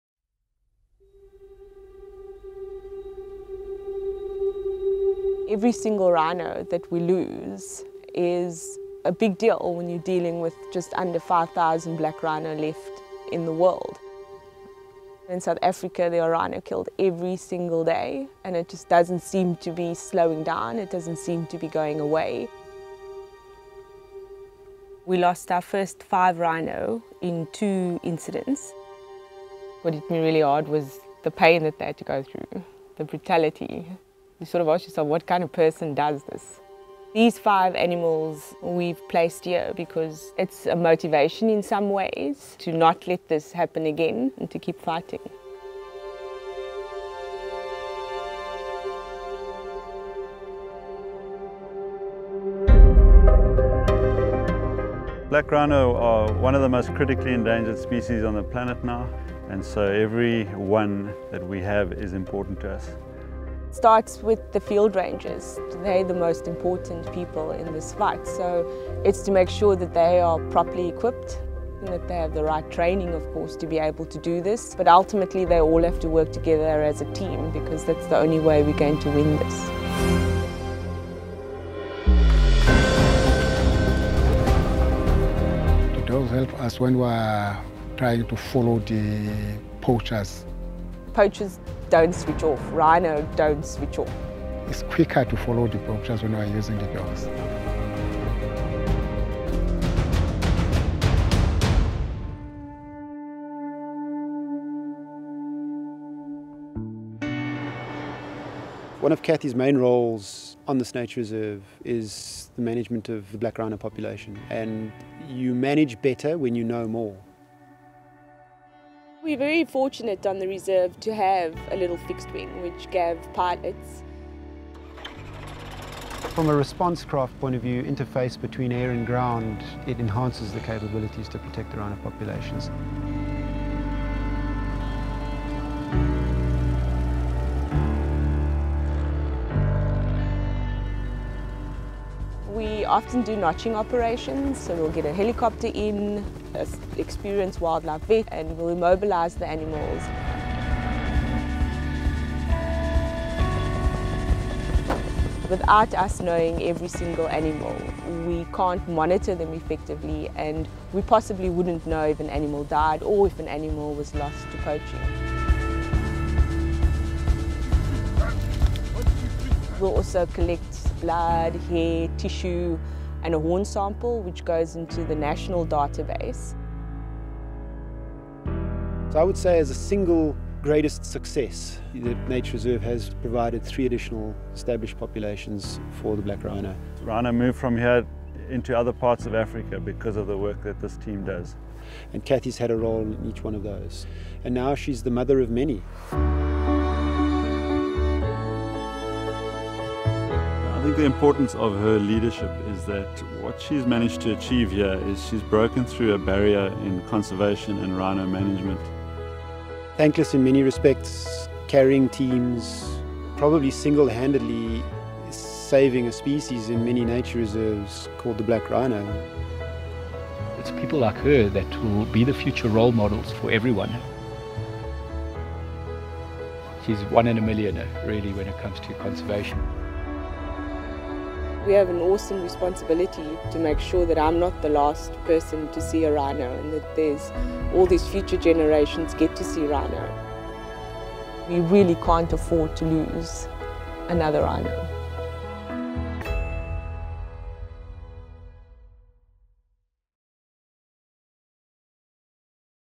Winner of the 2016 Tusk Award for Conservation in Africa - Cathy Dreyer
Cathy started her career over 17 years ago at the age of 22 with South African National Parks as a conservation student, assisting with the capture of black rhino. This became a turning point in her life as she developed a deep, lifelong passion for the species (Cathy calls it a “slight obsession”), which has shaped her career in conservation.

Coming from humble roots and starting a career at the bottom of the ladder, Cathy’s perseverance and focus built a career which graphically illustrates that being disadvantaged is not a barrier to achieving great heights, not only in conservation but also in life itself. On hearing about her nomination, Cathy said: “It has been incredibly humbling and I am truly honored to have been nominated for this award.”

Cathy’s success has been totally through her own efforts and as a woman she has faced greater challenges than most. She admits that: “I have always been driven by my work and family has had to take a back seat for a number of years.” Crucially, Cathy has helped to bring people working in conservation together, principally in a number of national and international rhino translocations.  Her response to the nomination shows her overarching modesty: “This nomination is as much for the people who have inspired me, taught me what I know and continue to motivate me daily.”

Cathy’s work has led directly to the establishment of new black rhino populations and in South Africa, the country with the highest population of rhino – yet facing the highest rate of poaching – Cathy offers the potential to provide vital protection for this species far into the future. Behind her success is her approach to life: “the harder the battle the sweeter the victory”.

Cathy Dreyer is a clear example of the commitment and future of young African leadership in Conservation in Africa: “I am driven by the need to feel that I am making a difference and contributing meaningfully to conservation.